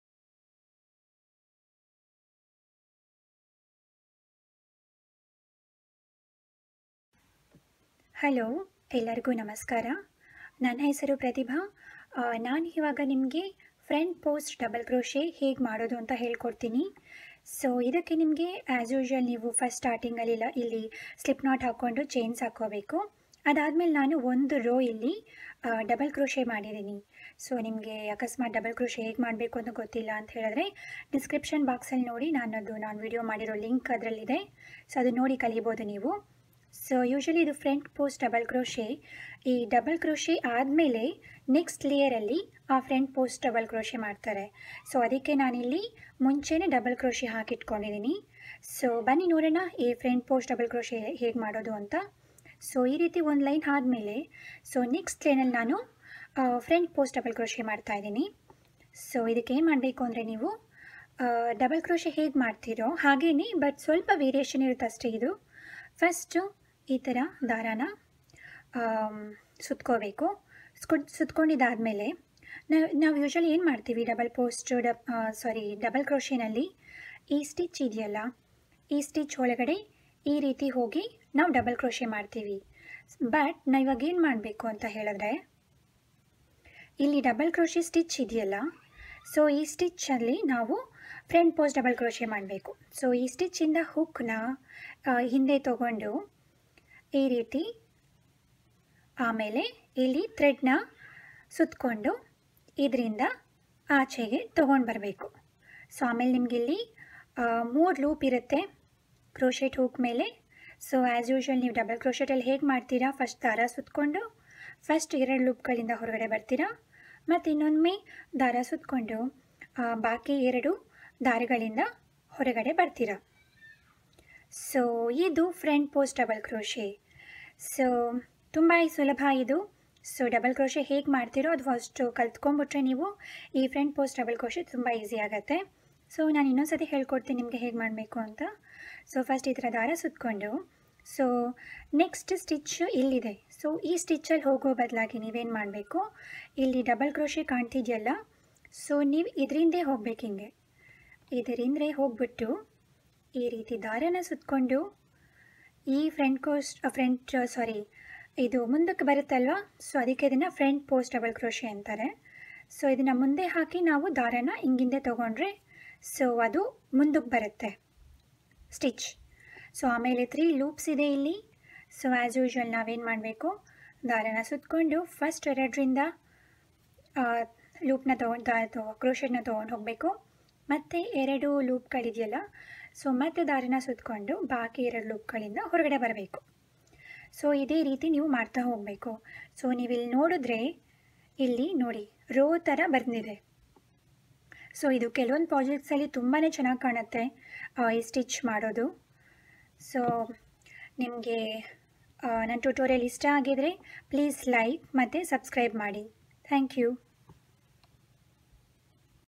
Front Post Double Crochet in Kannada - Crochet for beginners
You will learn about how to do Front Post Double Crochet in Kannada